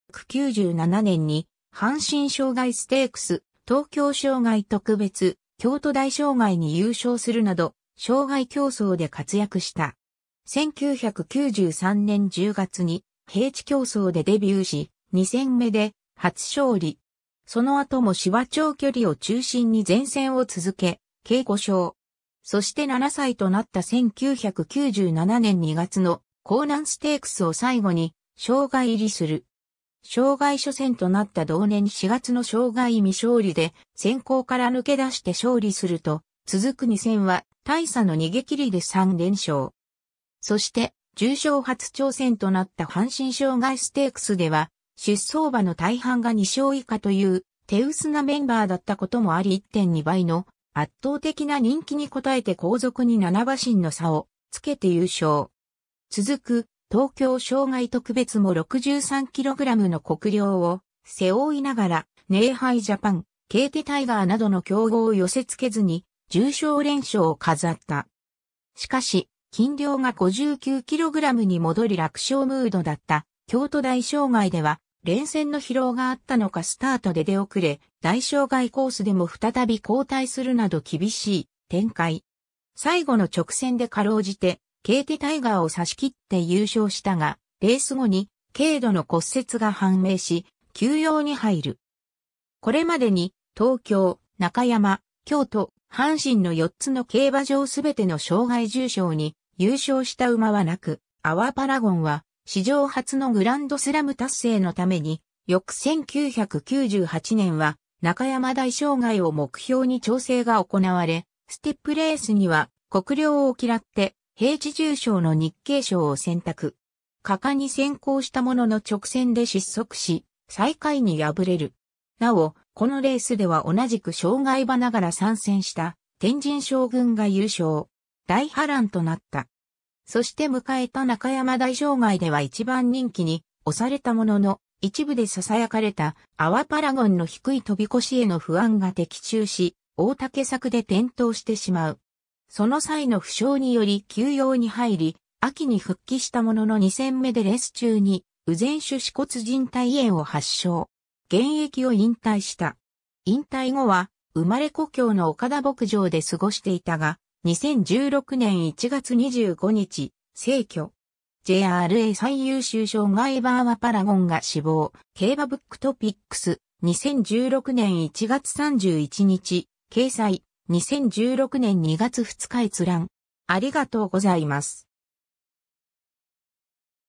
アワパラゴン
アワパラゴン (Our Paragon) とは日本の競走馬である。
1997年に阪神障害ステークス、東京障害特別、京都大障害に優勝するなど障害競走で活躍した。
 1993年10月に平地競走でデビューし、2戦目で初勝利。
その後も芝長距離を中心に善戦を続け計5勝。
そして7歳となった1997年2月の甲南ステークスを最後に障害入りする。
 障害初戦となった同年4月の障害未勝利で先行から抜け出して勝利すると、続く2戦は大差の逃げ切りで3連勝。
そして重賞初挑戦となった阪神障害ステークス（秋）では出走馬の大半が2勝以下という手薄なメンバーだったこともあり1.2倍の圧倒的な人気に応えて後続に7馬身の差をつけて優勝。
続く東京障害特別（秋）も63kgの酷量を背負いながらネーハイジャパン、ケイティタイガーなどの強豪を寄せ付けずに重賞連勝を飾った。
しかし、斤量が59kgに戻り楽勝ムードだった京都大障害（秋）では、連戦の疲労があったのかスタートで出遅れ、大障害コースでも再び後退するなど厳しい展開。
最後の直線で辛うじてケイティタイガーを差し切って優勝したが、レース後に軽度の骨折が判明し休養に入る。
 これまでに東京、中山、京都、阪神の4つの競馬場全ての障害重賞に優勝した馬はなく、アワパラゴンは史上初のグランドスラム達成のために翌1998年は中山大障害を目標に調整が行われ、ステップレースには酷量を嫌って平地重賞の日経賞を選択。
果敢に先行したものの直線で失速し最下位に敗れる。
なお、このレースでは同じく障害馬ながら参戦したテンジンショウグンが優勝。
大波乱となった。
 そして迎えた中山大障害（春）では1番人気に推されたものの、一部で囁かれたアワパラゴンの低い飛越への不安が的中し大竹柵で転倒してしまう。
その際の負傷により休養に入り、秋に復帰したものの2戦目でレース中に右前種子骨靱帯炎を発症。
現役を引退した。
 引退後は生まれ故郷の岡田牧場で過ごしていたが、2016年1月25日逝去。
 ^ JRA最優秀障害馬アワパラゴンが死亡 - 競馬ブックトピックス、2016年1月31日掲載、2016年2月2日閲覧